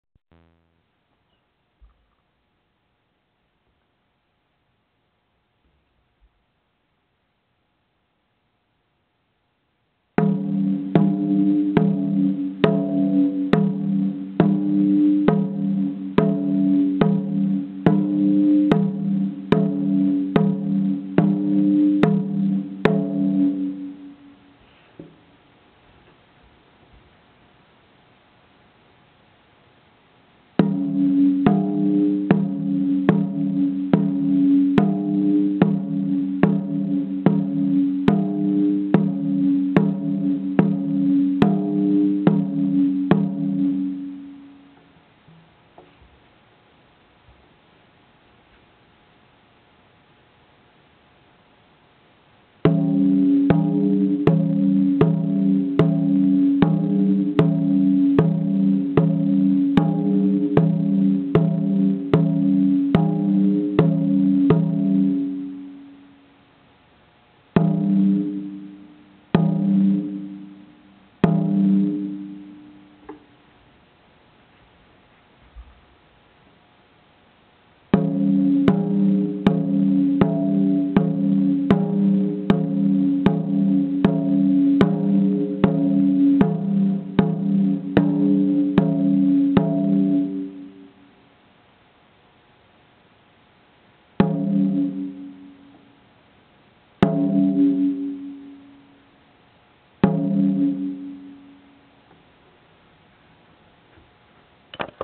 Lateral Katsugi Oke Daiko
An example of form for lateral katsugi okedo.

I'M SORRY!  My drum is facing the wrong direction in these videos and I didn't have time to re-do them.  The right side should be the higher pitch.